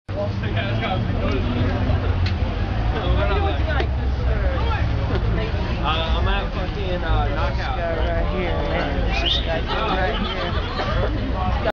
Hunter and Dr. Gonzo (Again)
More of Hunter S. Thompson and Dr. Gonzo strolling San Francisco streets on Halloween